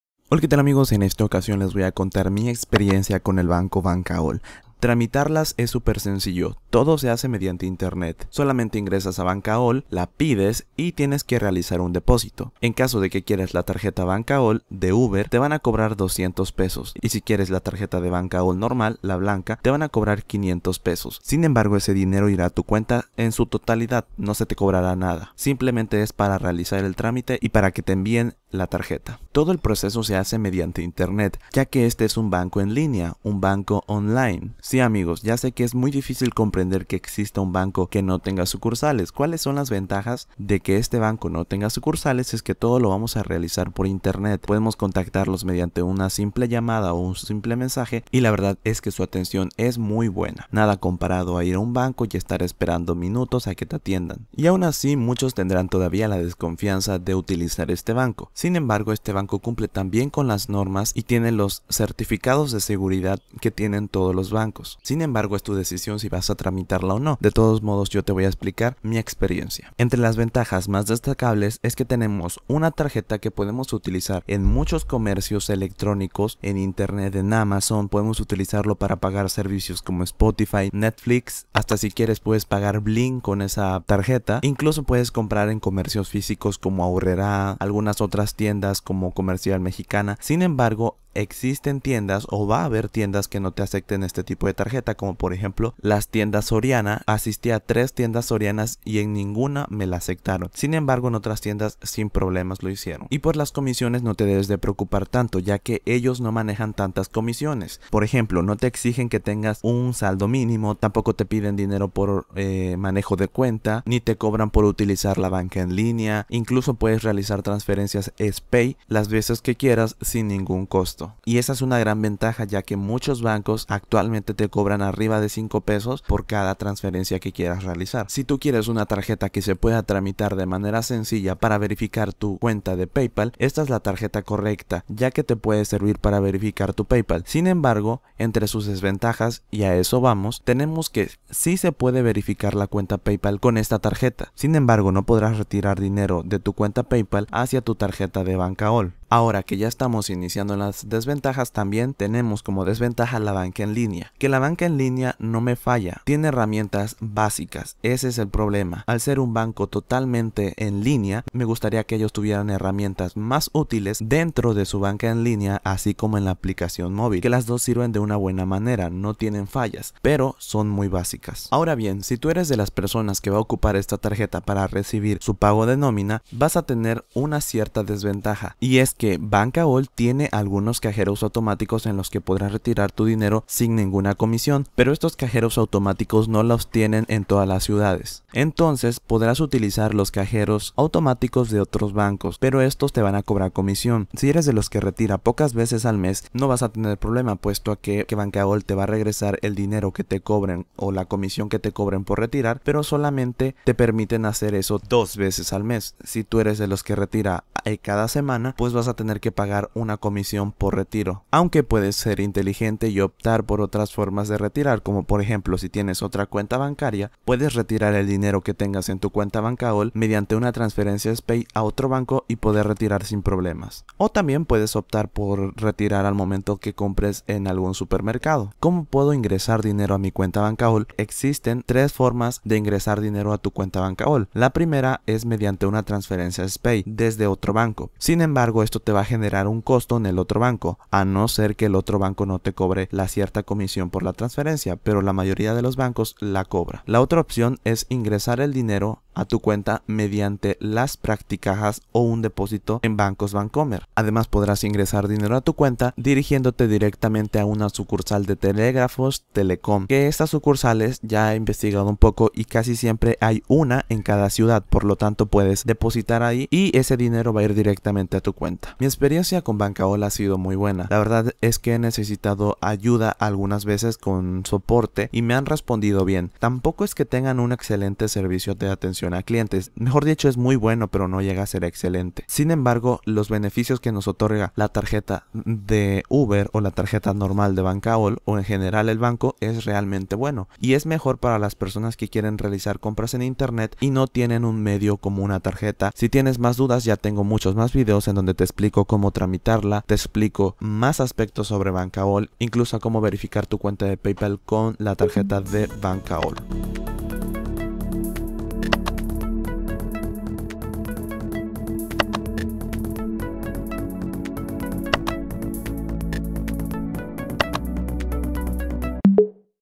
EXPERIENCIA con el primer BANCO ONLINE , sin sucursales: BANKAOOL [PRODUCTO DESCONTINUADO]
Mensaje IMPORTANTE:
ESTE VÍDEO YA NO ES VIGENTE. Hoy en día Bankaool es un banco con sucursales que opera en el estado de Chihuahua, actualmente NO ofrecen ni la tarjeta UBER ni los productos mencionados en este vídeo ni tampoco tienen relación a Banco Famsa y por ende los opiniones y/o datos mencionados en este vídeo NO representan al banco hoy en día. Conoce más sobre el nuevo Bankaool

Esta vez hablaré un poco de la experiencia que he tenido con Bankaool, el primer banco mexicano en ofrecer su producto financiero desde una plataforma online, no cuentan con sucursales. Solo cuentan con una oficina. El servicio a clientes se hace en Linea, si quieres abrir una cuenta bancaria solo bastará entrar a su web y pedirla. Es sencillo, y rápido. Aquí te decimos si es que vale la pena o no los productos que ofrecen.
Yo tengo la tarjeta Uber de Bankaool, que funciona de la misma manera que la Bankaool Blanca.

Por cierto, tuve un problema de edición en el minuto 3:25 en donde tuve que encimar un audio medio raro en donde digo "dos" ya que en el audio original dije "tres". En fin, perdón por el error.

►►►ENLACES RECOMENDADOS◄◄◄
Comprar gratis en Amazon, ¿Se puede?:

►►►►►►CRÉDITOS ◄◄◄◄◄◄
Vibe Tracks
Autor del vídeo: Isaac David Bonilla